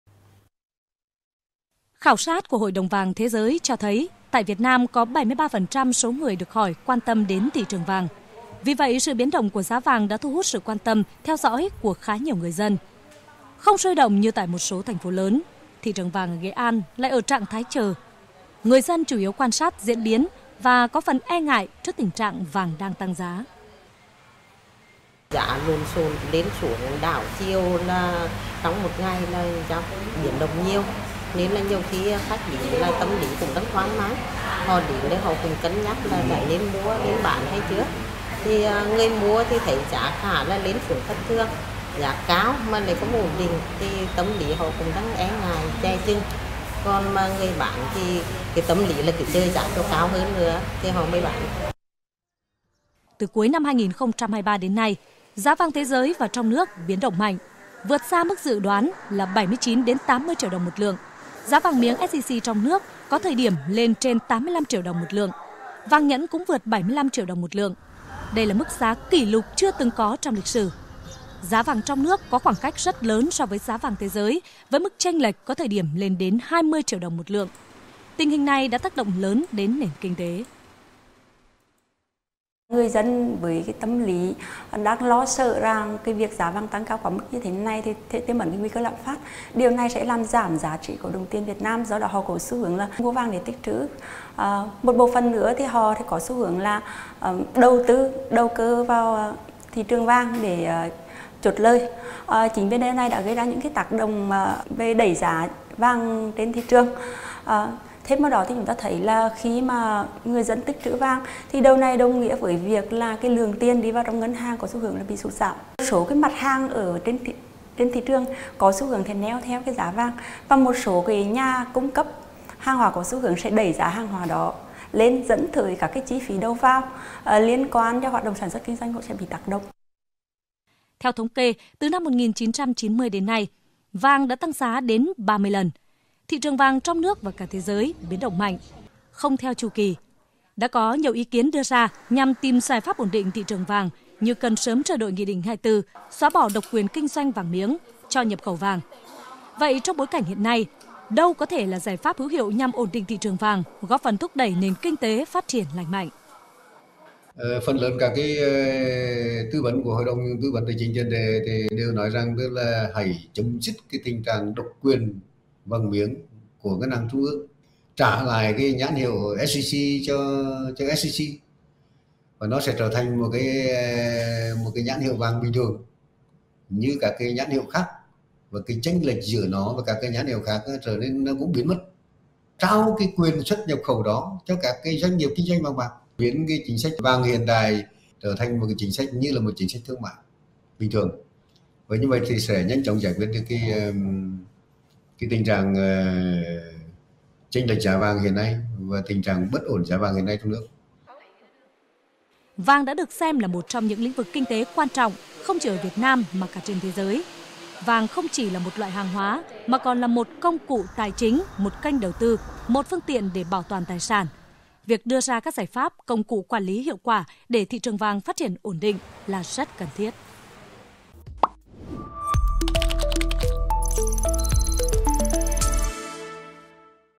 Giải pháp bình ổn, quản lý thị trường vàng?
Vàng được xem là một kênh trú ẩn an toàn và kim loại quý này được nhiều nhà đầu tư lựa chọn. Tuy nhiên, trong thời gian qua, thị trường vàng có nhiều biến động. Giá vàng liên tục lập những đỉnh mới, lên xuống thất thường. Điều này đã đặt ra nhiều thách thức đối với công tác quản lý, từng cá nhân đầu tư cũng như cả thị trường.